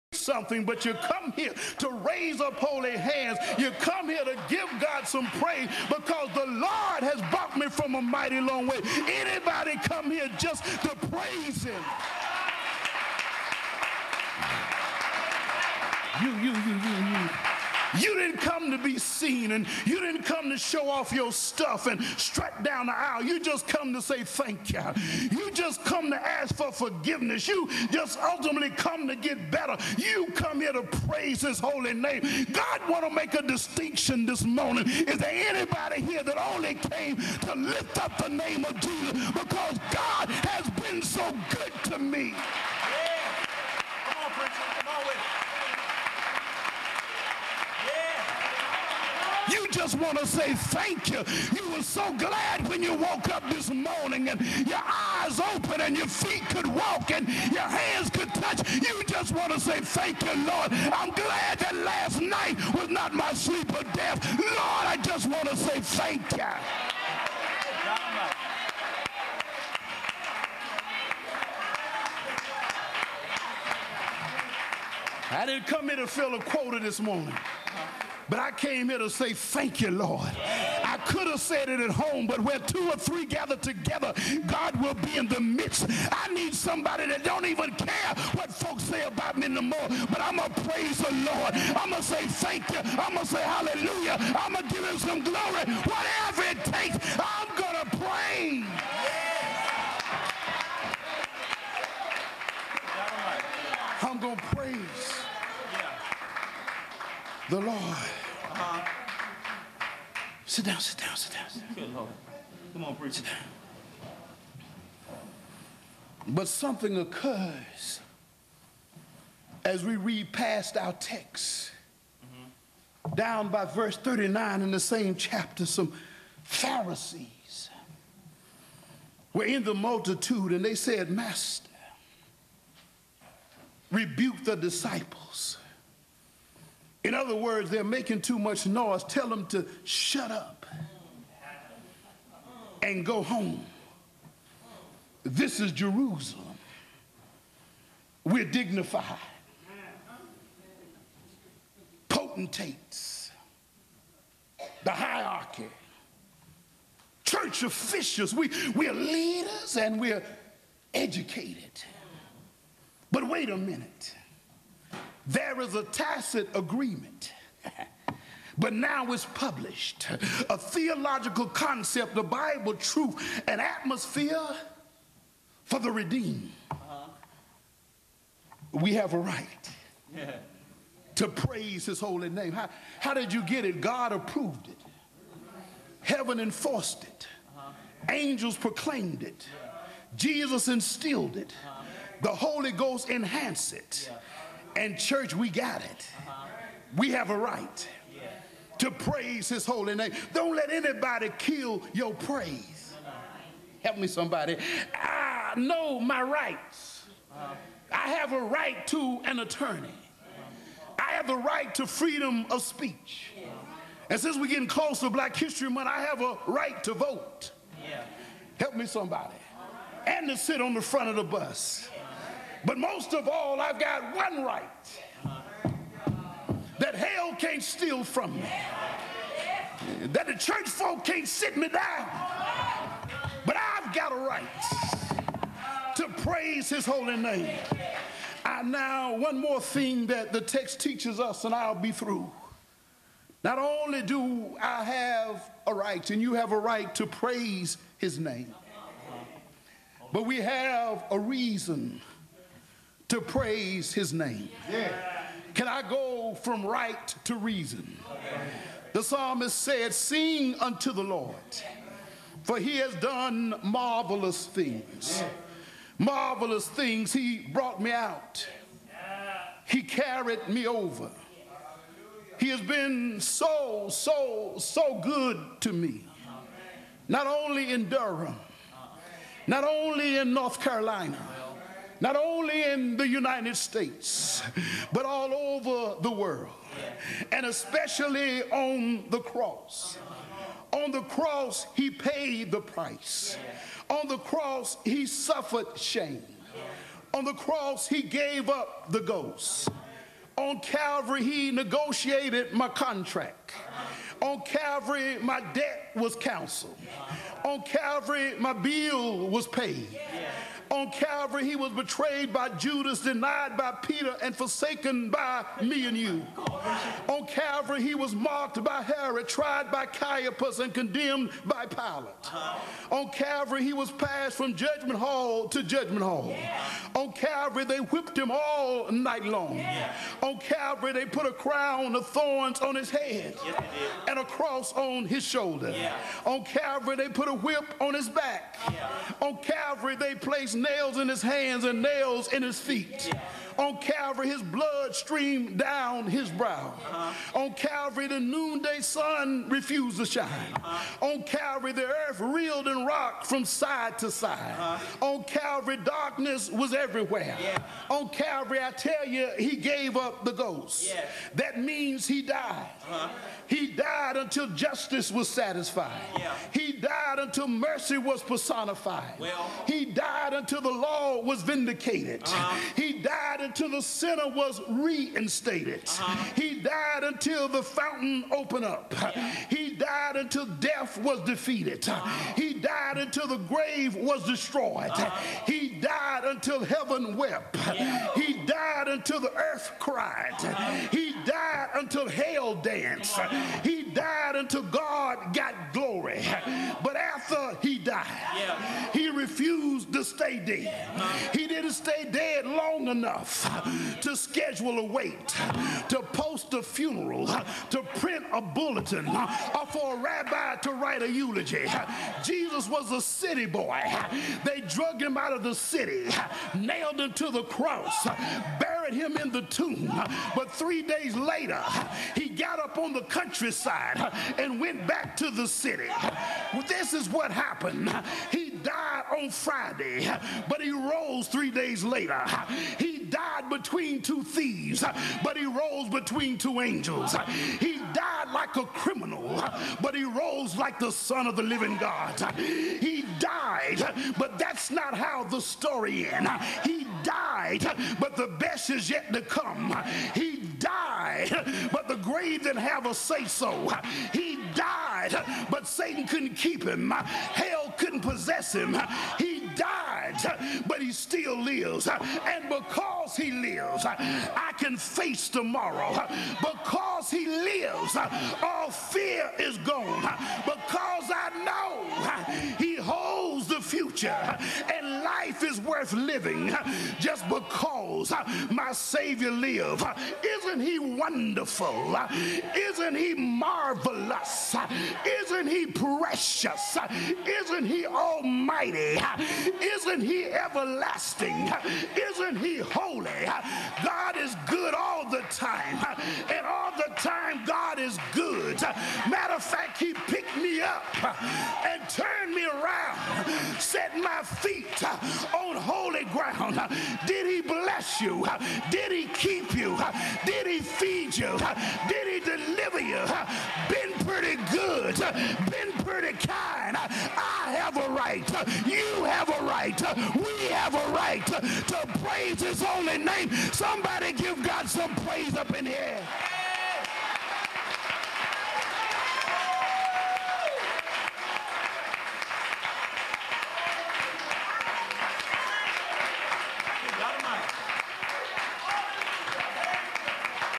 Rev. Dr. Glenn R. Davis - Sermon 1 - Part 1
This is Rev. Dr. Glenn R. Davis preaching at New Bethel Baptist Missionary Church.